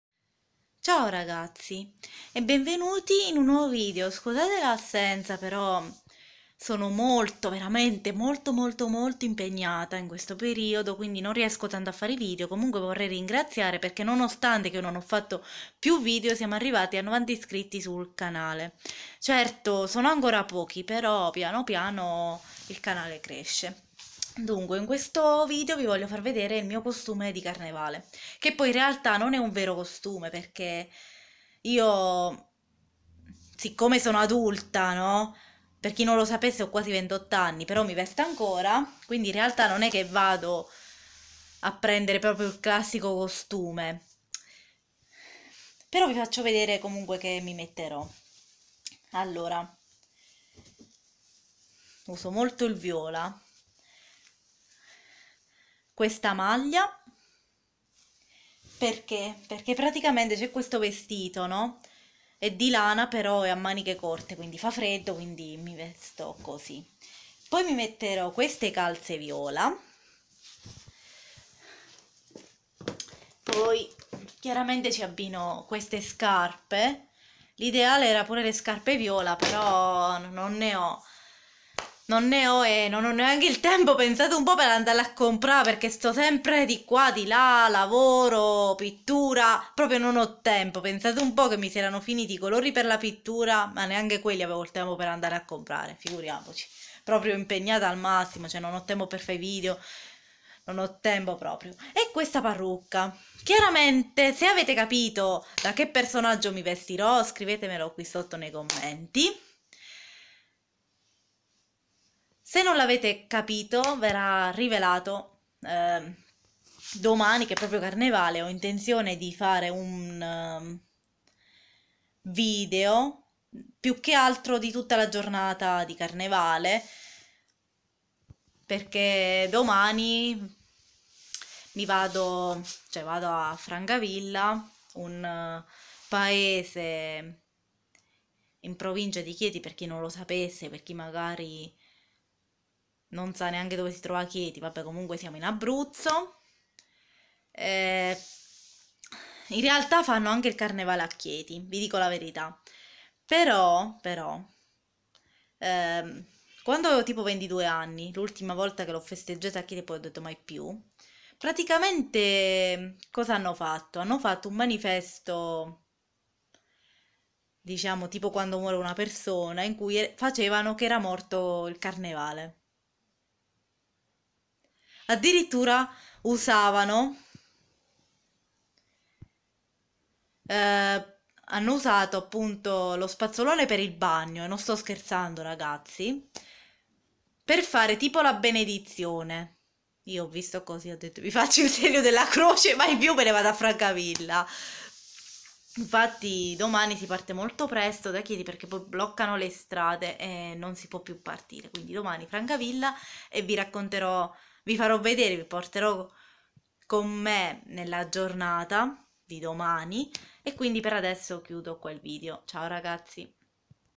Vi mostro il mio costume di Carnevale
Vediamo se riuscite ad indovinare da quale personaggio dei "my little pony" mi travestirò stavolta...